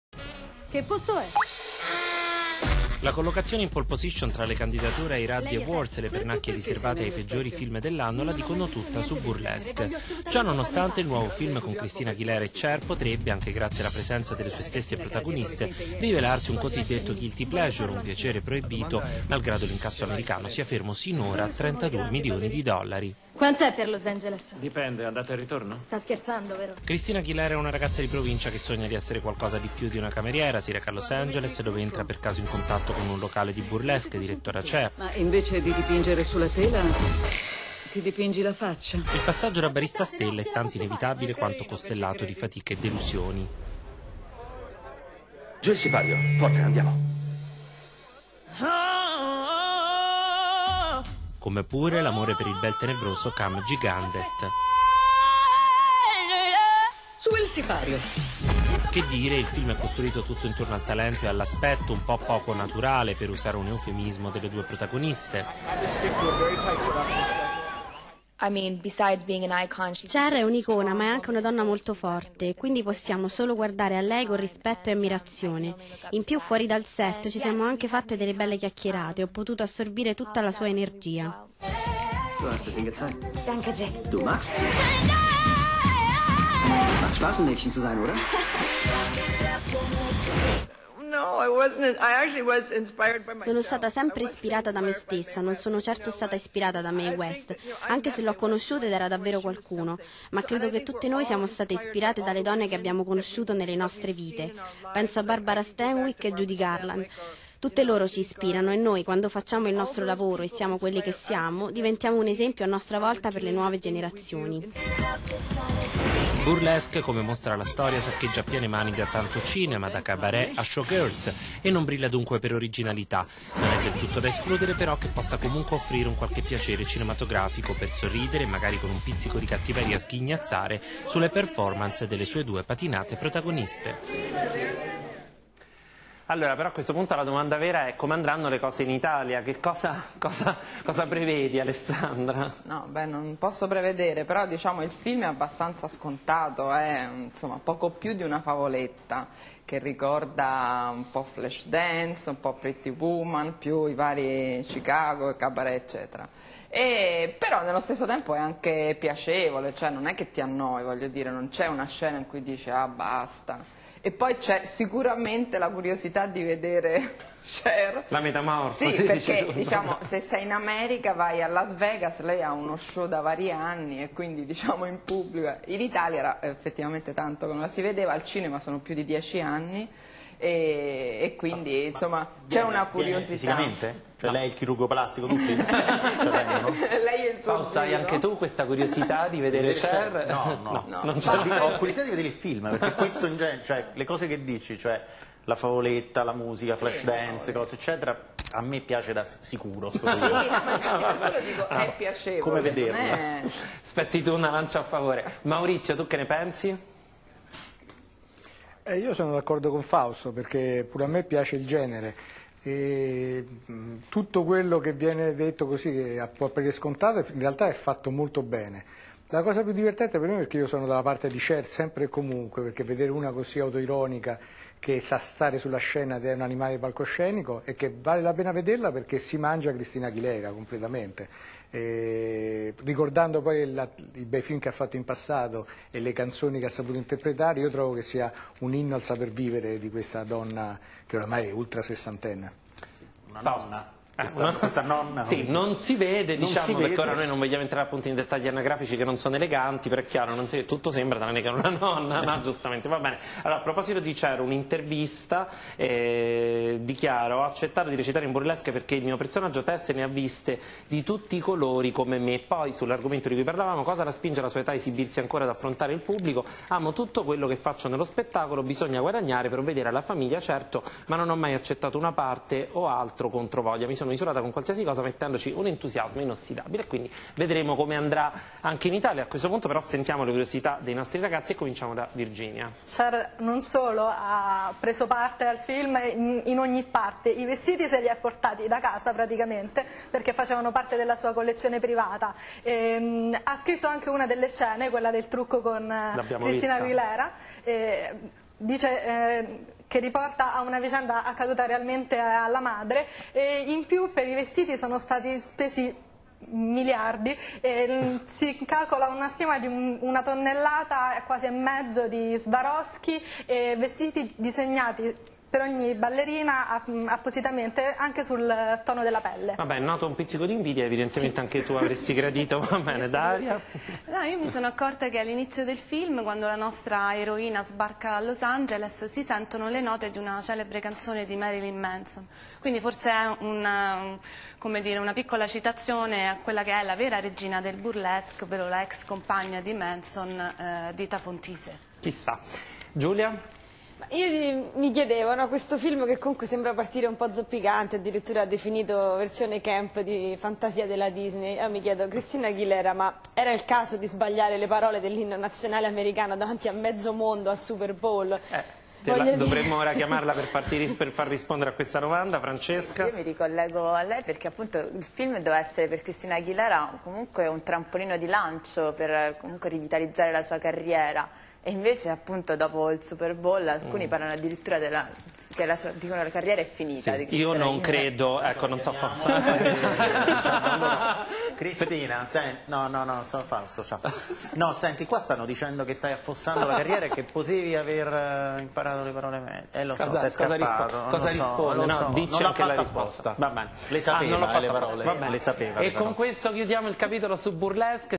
Burlesque (Christina Aguilera,Cher)
Christina Aguilera e Cher sono le protagoniste del musical Burlesque, che tenta la carta europea dopo l'accoglienza tiepida della critica americana. E mentre i fan delle due star attendono l'arrivo in sala, il film fa il pieno di candidature ai Razzie Awards, gli anti-Oscar che decretano le peggiori performance dell'anno.